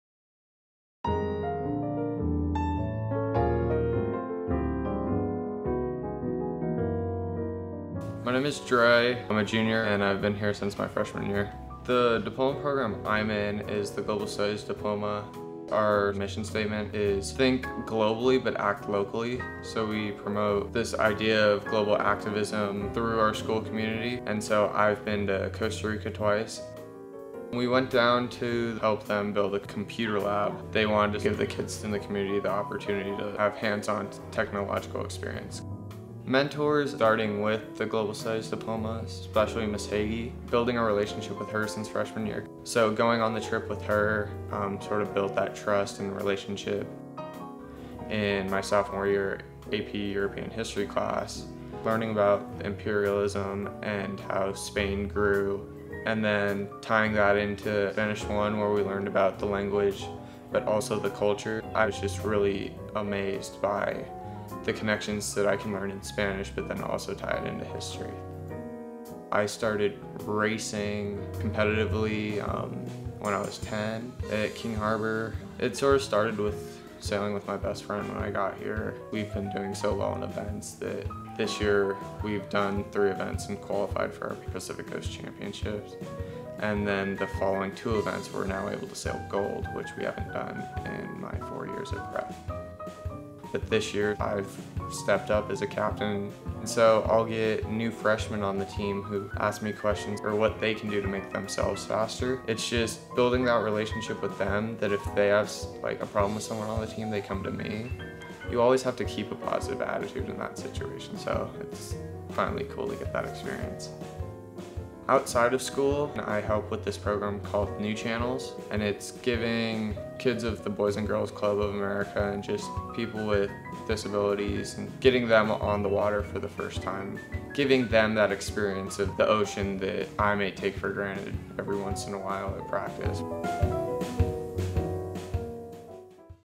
Student Spotlights: Drea Keswater
Drea is a candidate for the specialized diploma from the Global Studies Program at RHP and Ren. Apart from his academic work, he is an important member of the schools' sailing team.